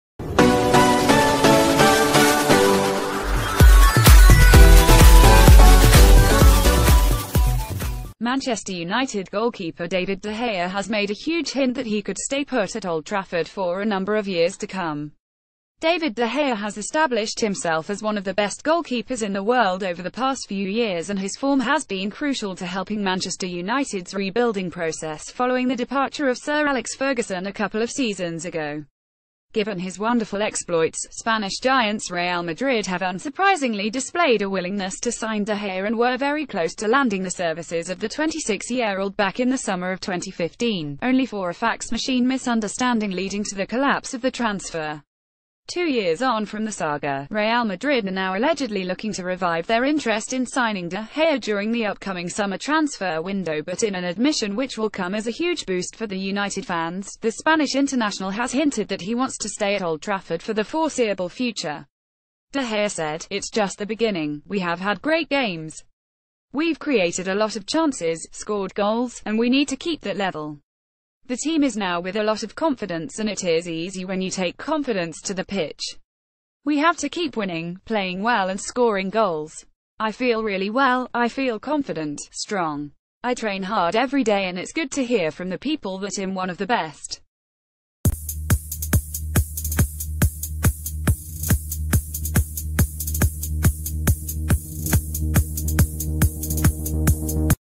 ‘it’s just the beginning’ – manchester united star makes huge admission about future
Manchester United goalkeeper David De Gea has made a huge hint that he could stay put at Old Trafford for a number of years to come. David De Gea has established himself as one of the best goalkeepers...